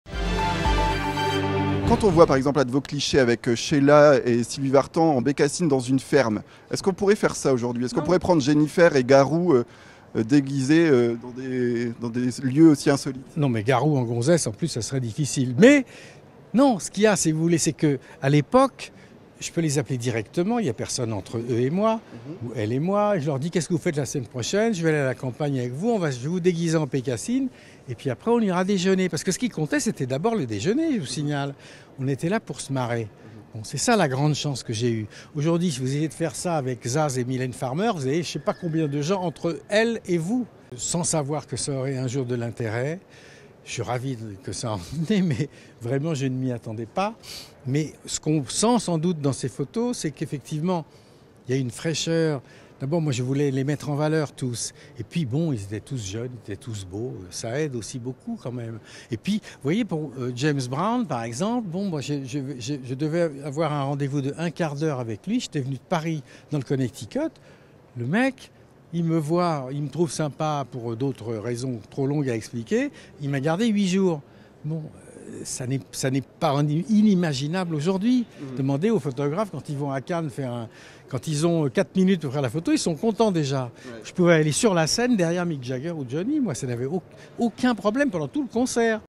Le célèbre photographe Jean-Marie Périer à la foire Art Up!
Après l’art et l’architecture, pour sa 12ème édition, la foire d’art contemporain lilloise Art Up! a décidé de poser son regard sur la photographie.

L’évènement se tient jusqu'à dimanche à Lille Grand Palais. Cette année la photographique et sa pratique sont mis à l’honneur sur le thème « La photographie au cœur des créations artistiques aujourd’hui. »

Le tout avec l’événement Fotofever, qui met en avant des artistes de renom, comme la venue, hier soir, de Jean-Marie Périer, célèbre photographe de musiciens et acteurs. Dans les années 60, il a immortalisé de nombreuses célébrités, de Françoise Hardy jusqu’aux Rolling Stones. Il était de passage afin de tenir une conférence à l’occasion de cet événement culturel. Un artiste qui possède une liberté et une imagination qui lui a permis de sortir des clichés originaux.